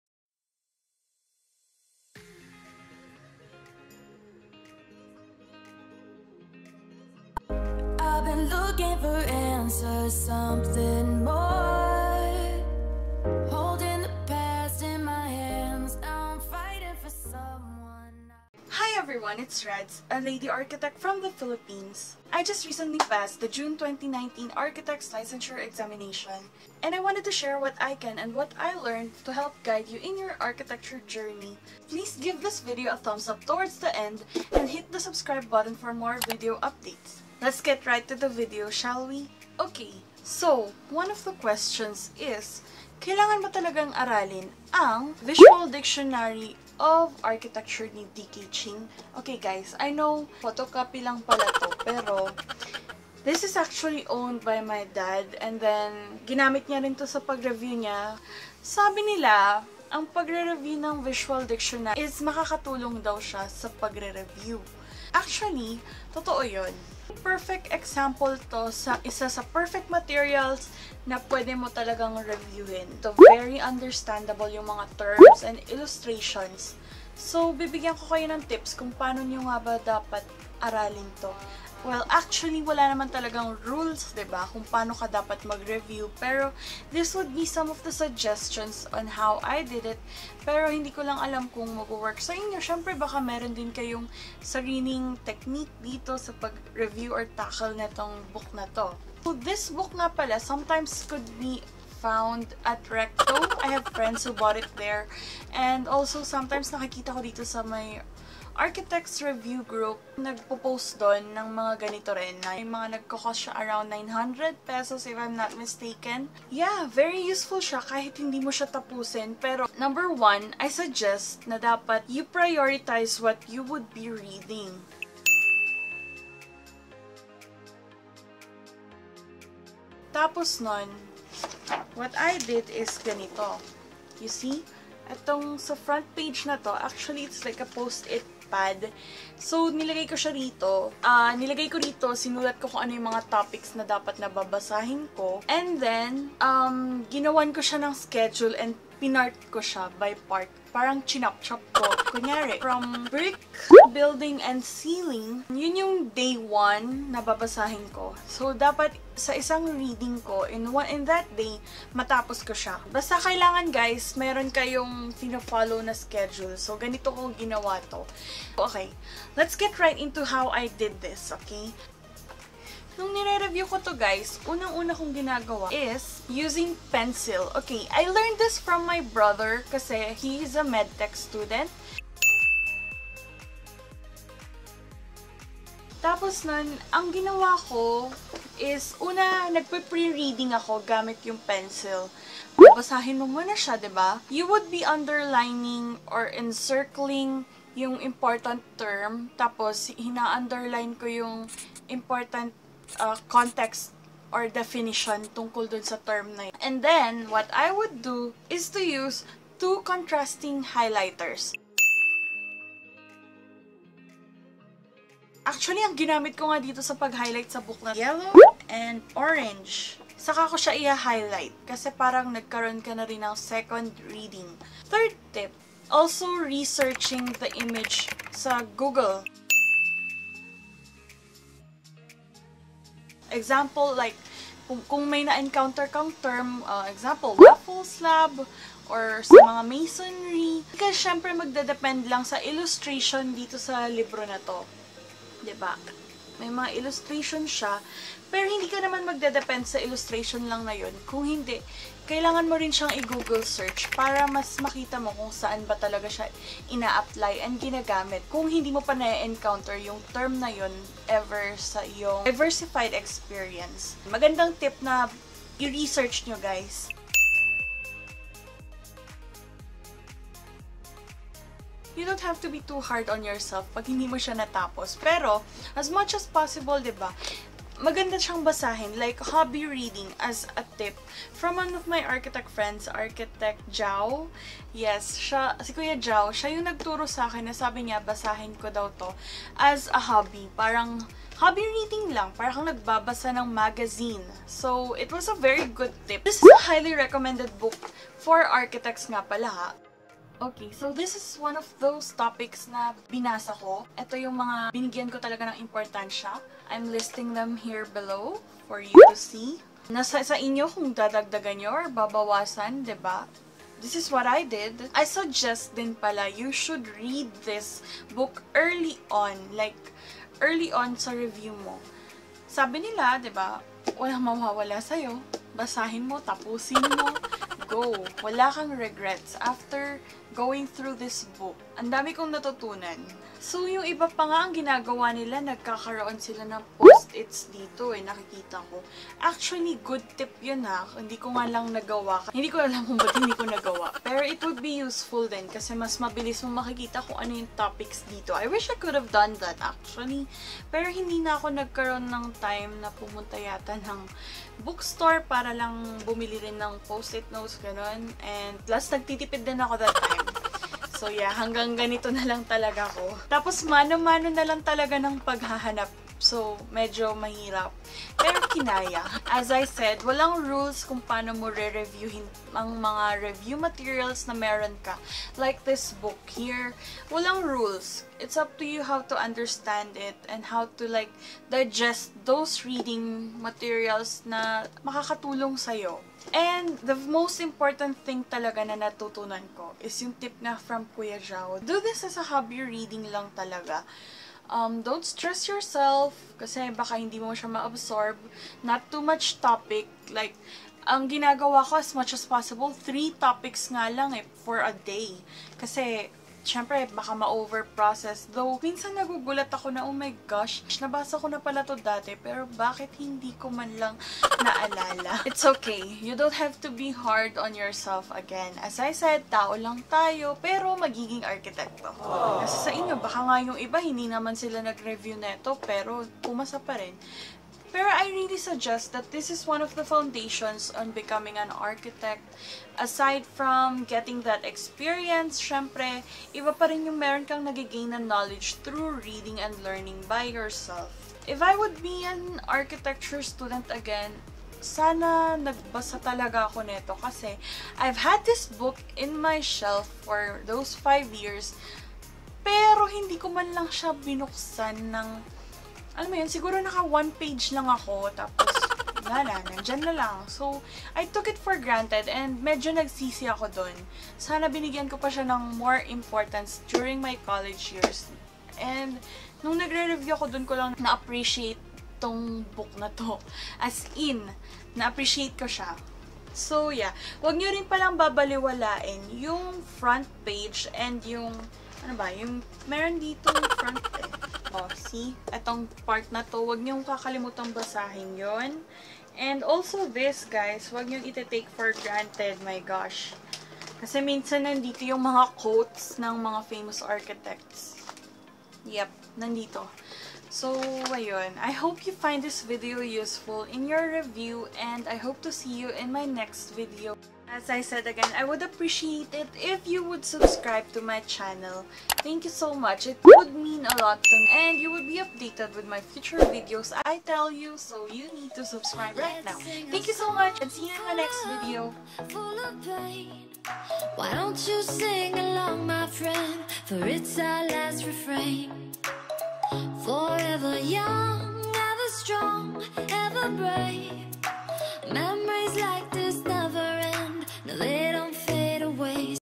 REVIEWING FOR THE ALE | A Visual Dictionary of Architecture by Francis D.K. Ching
This book is just one of those review materials that helped me a lot in reviewing for the Architects Licensure Exam. As a suggestion from a good friend, Ar. Jao Abenir, maximize using this material by "leisure reading" to enjoy reviewing the material even more. Good Luck future Architect!!!

I'll be sharing future videos about review tips too. I appreciate it and it meansss a lot!

Here's what I read:
ARCH
BRICK
BUILDING
CEILING
CERAMIC
CHURCH
COLUMN
CONCRETE
CONSTRUCTION
DESIGN
DOME
DOOR
DRAWING
ELECTRICITY
ELEVATOR
FASTENING
FIREPLACE
FIRE SAFETY
FORTIFICATION
FLOOR
FOUNDATION
FRAME
GLASS
HARDWARE
HEAT
HOUSE
JOINERY
LIGHT
MASONRY
MATERIAL
MOISTURE PROTECTION
ORDER
ORNAMENT
PAINT
PLASTICS
PLATE
PLUMBING
REINFORCED CONCRETE
ROOF
ROOM
SITEWORK
SHELL
SOIL
SOUND
STAIRS
STONE
STRUCTURE
SURVEY
TEMPLE
THEATER
TRUSS
VAULT
WALL
WINDOW
WOOD

Good Luck future Architect! I'll be sharing future videos about review tips too. I appreciate it and it meansss a lot!